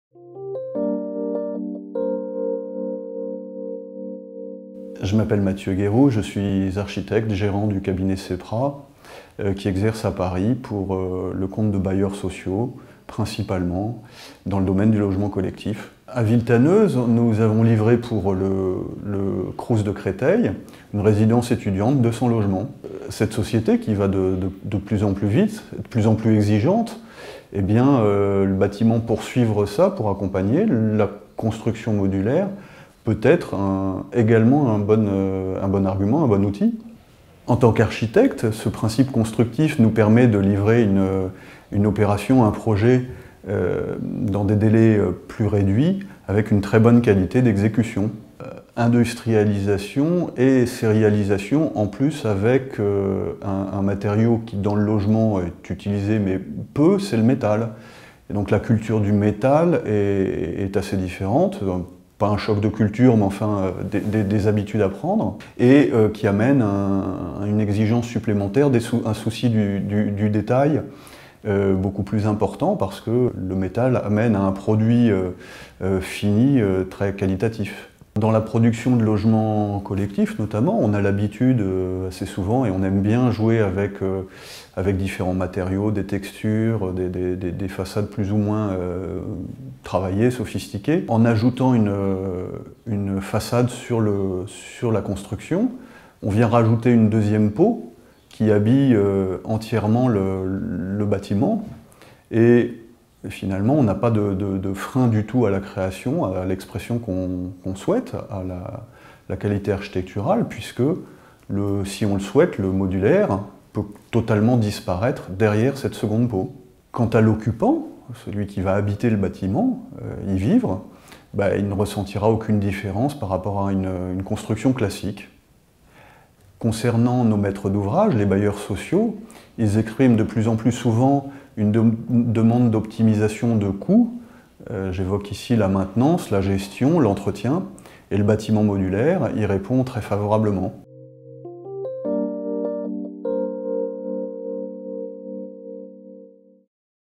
Interview de Matthieu GUEROULT (SEPRA ARCHITECTES)
Interview de Matthieu GUEROULT, architecte gérant de SEPRA ARCHITECTES.
Réalisation CITEDEN COUGNAUD :  Résidence étudiante Crous Paris XIII - Villetaneuse [93]